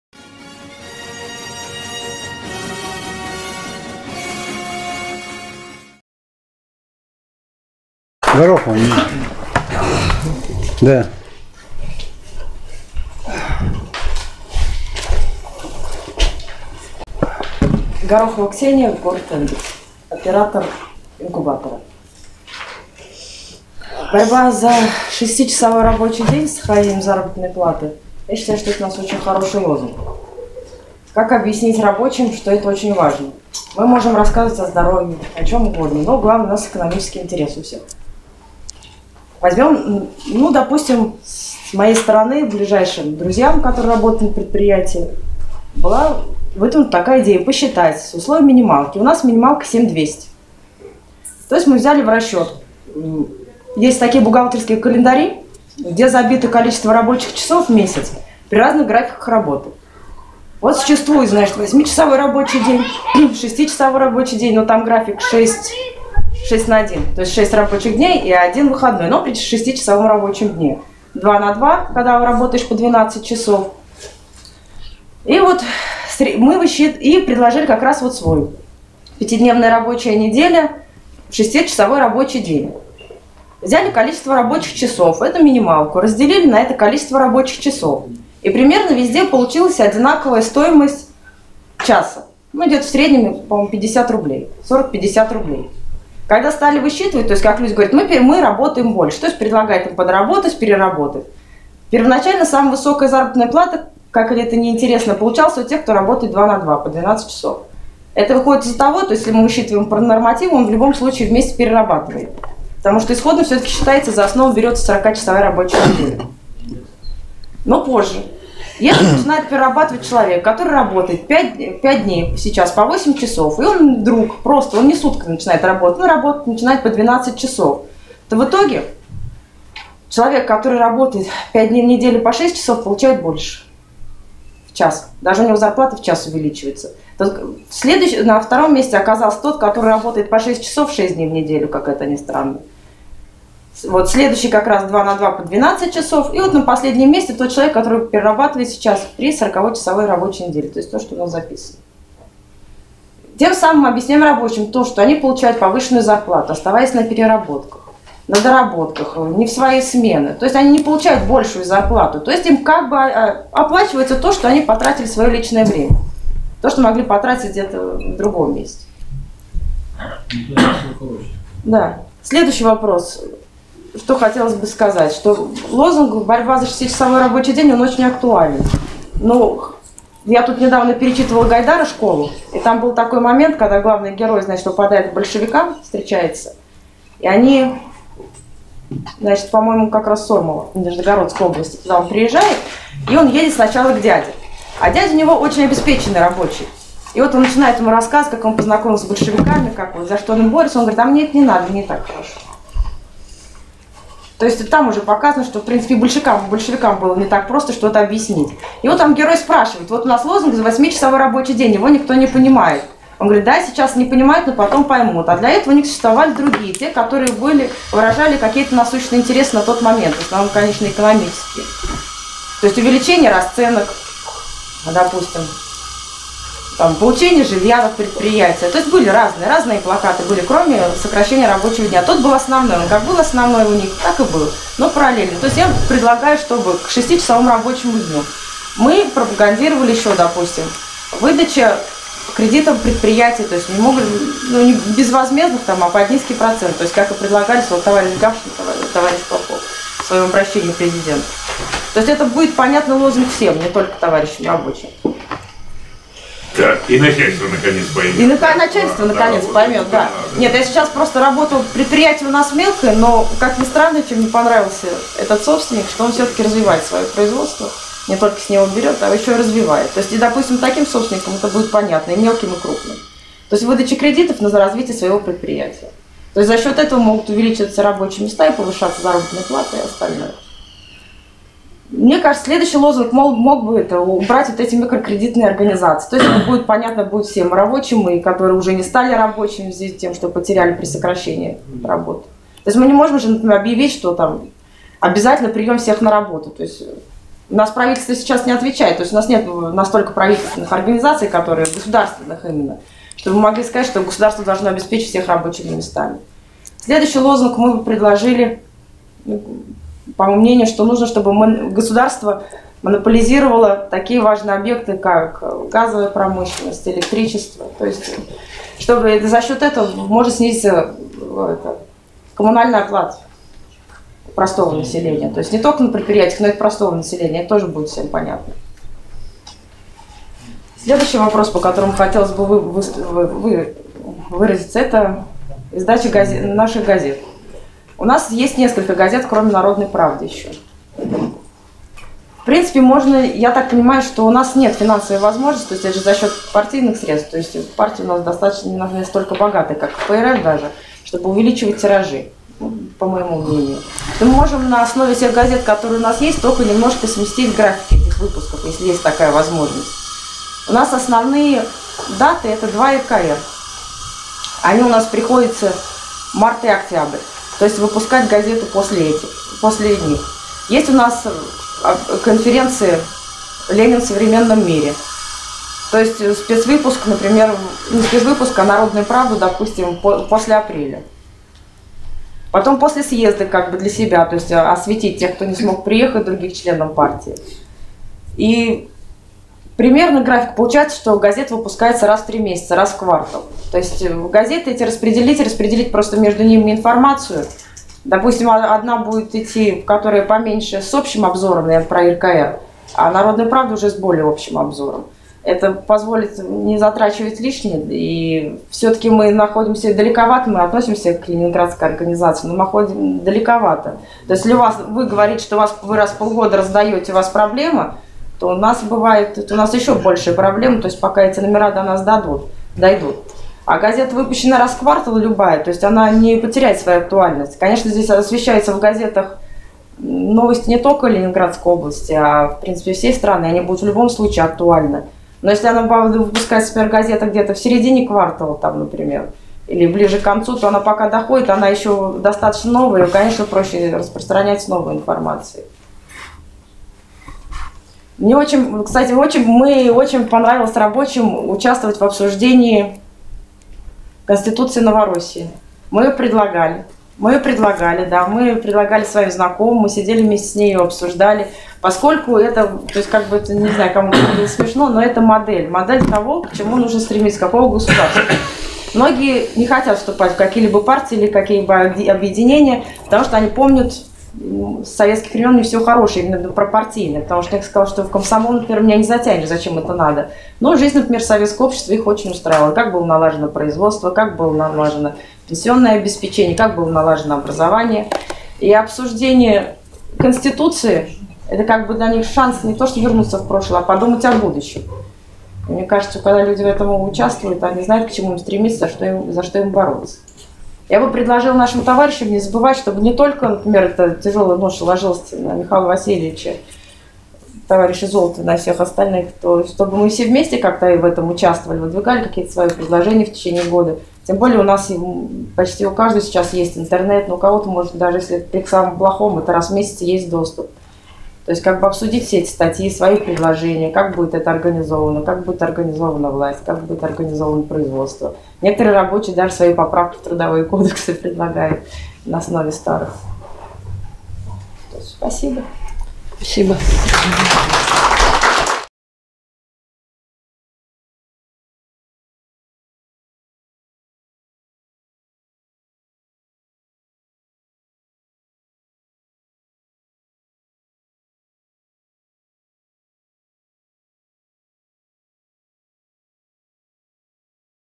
XXIV Съезд Рабочей партии России (10 и 11 июня 2017 г.). К.В.Горохова.
XXIV Съезд РПР (10 и 11 июня 2017 г.). К.В.Горохова.
Оператор инкубатора, г. Энгельс.

Желающие помочь Фонду Рабочей Академии могут перечислить пожертвования на карту Сбербанка:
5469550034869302